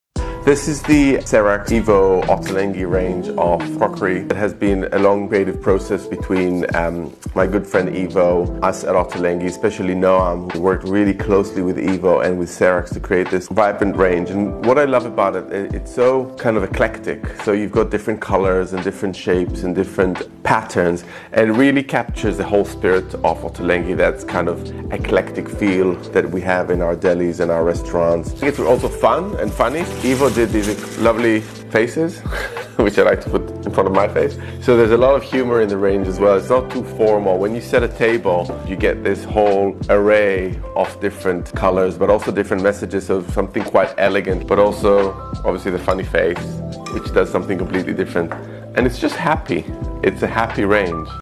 A FEAST For The Eyes
British chef Ottolenghi joins forces with Italian artist Bisignano in a stunning tableware range for Serax. The result is a gorgeous collection, characterised by the impulsive gesture of an ‘O’ shaped brush stroke, blending vegetable motifs with dazzling splashes of bright colour – a true FEAST for the eyes.